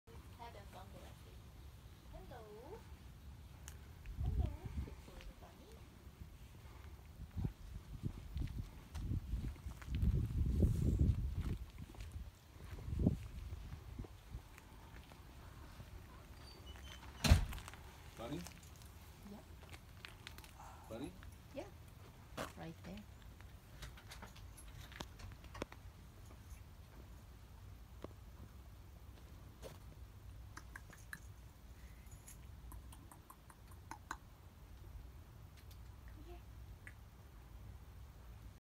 Wild Rabbit in the garden
Wild Baby Bunny Action-this wild baby rabbit seems to have adopted us! She roams the garden and helps is to weed it by eating the weeds .  She is a cute bunny too.  I hope you enjoy our funny bunny videos and come back each month to see more!  Bunnychan is a result of animal rescue, she was found in the family garden and we feed her sometimes.  Other than that, she is a totally wild baby rabbit.